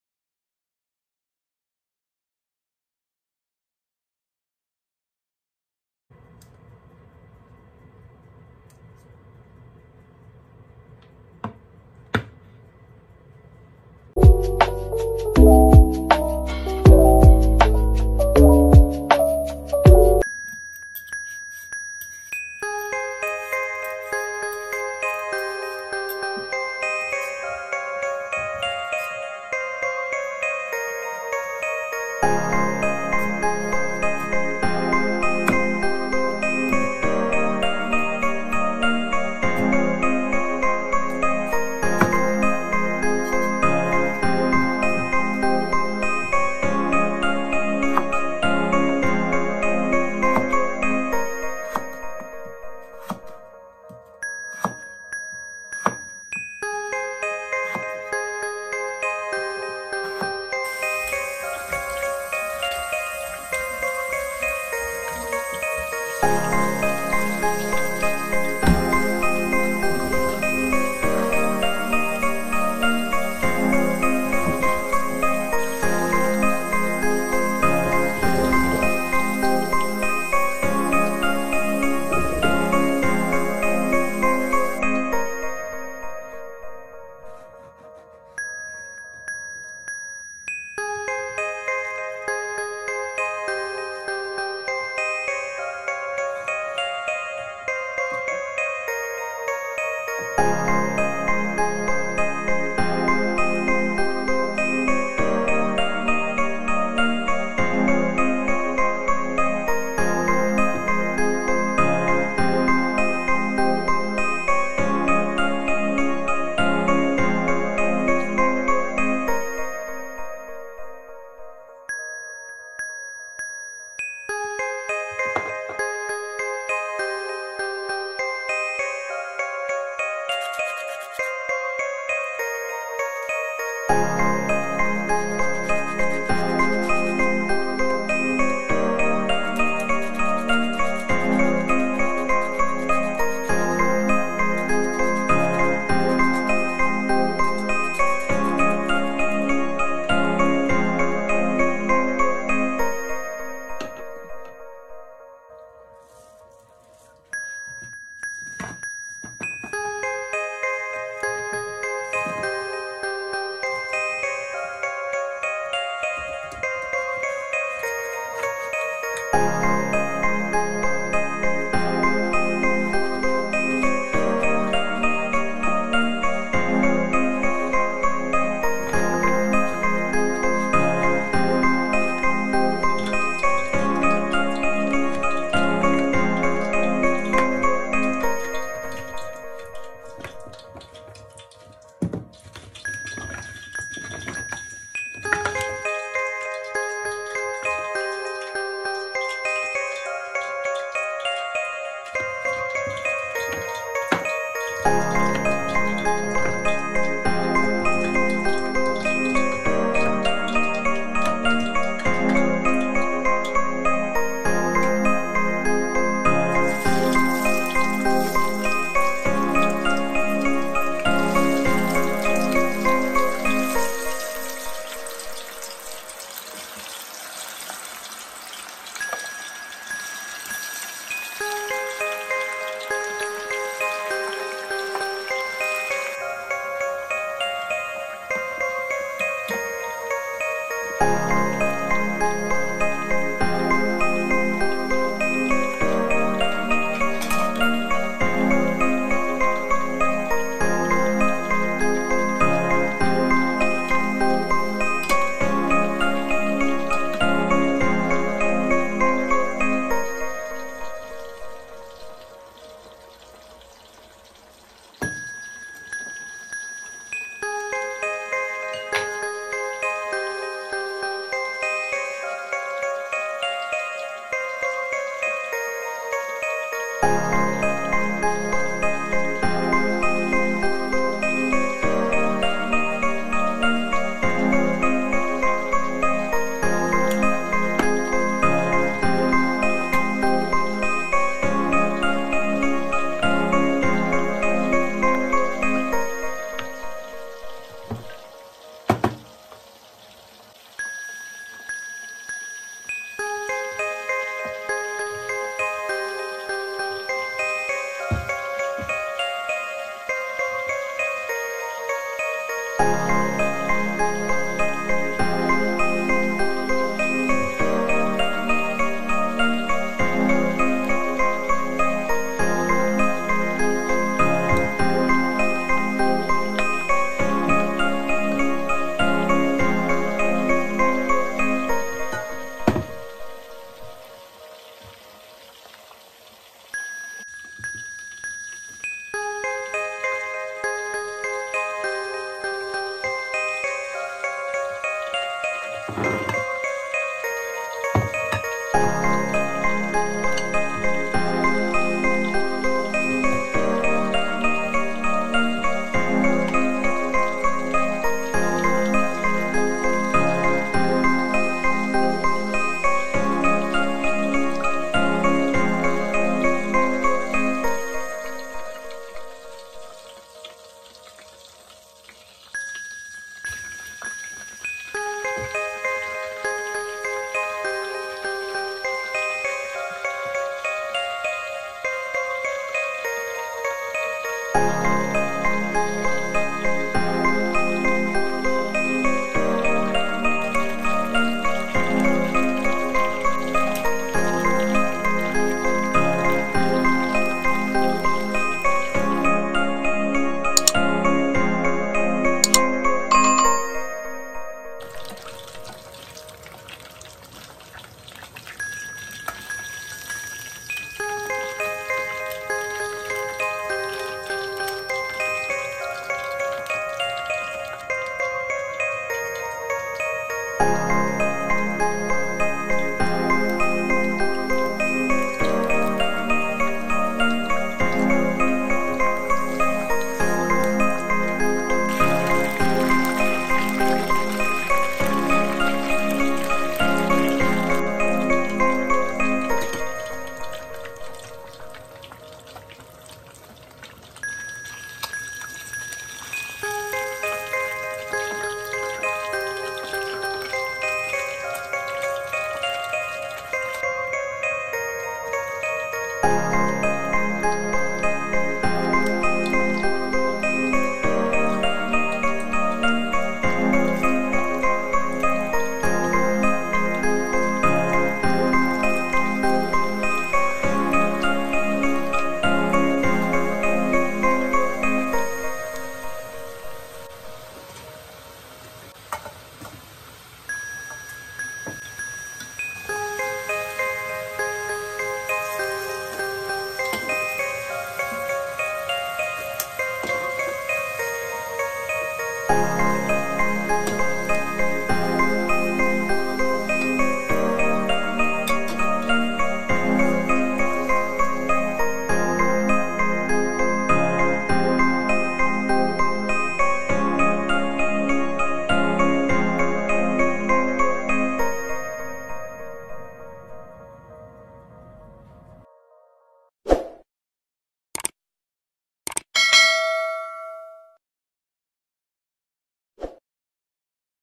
Made Fish and Chips (78th)フィッシュ&チップスの作り方#isenorma#madefish#and chips
England’s famous food using cod fish or any white fish.It’s crispy when fried and together with potatoes, they make a superb taste.